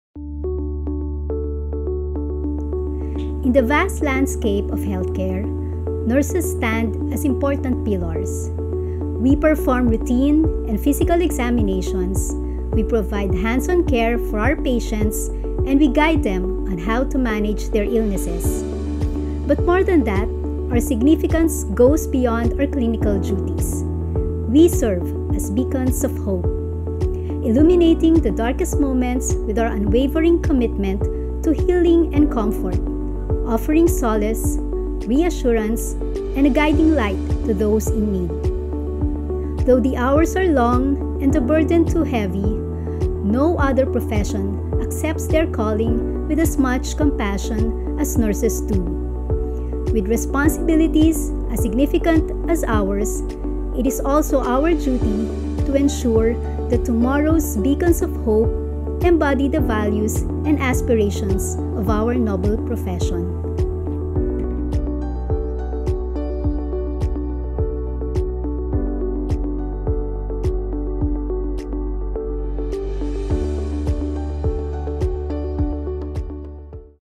Beacons of Hope (International Nurses Day)
May 12th marks the International Nurses Day, and in honor of the remarkable dedication of nurses worldwide, we wanted to show our appreciation by highlighting the crucial role they play in the medical field.

Let us reaffirm our commitment to supporting the advancement of nursing education and the invaluable contributions of nurses worldwide.

Credits:
Illustrated and Animated by Lemuel Pablo
Narrated by Jellen Alegre
Produced by Mary Joy Olazo